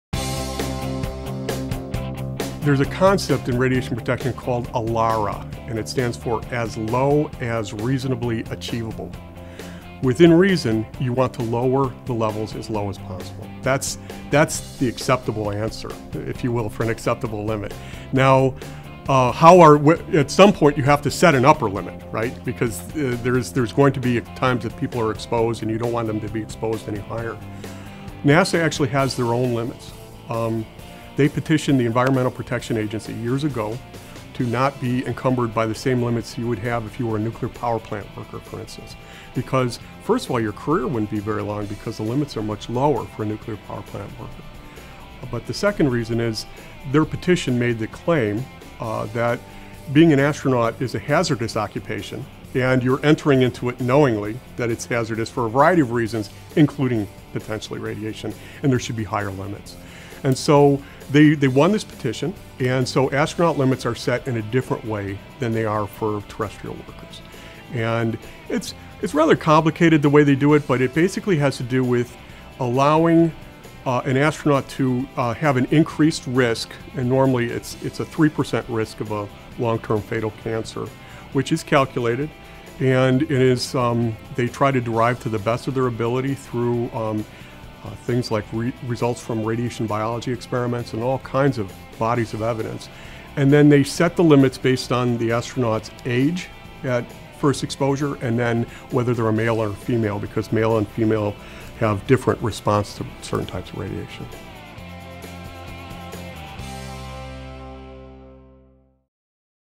What Are Acceptable Limits of Radiation?
Brad Gersey, lead research scientist at the Center for Radiation Engineering and Science for Space Exploration, or CRESSE, at Prairie View A&M University, describes the legal and acceptable limits of radiation exposure and the effort to keep those limits as low as reasonably achievable.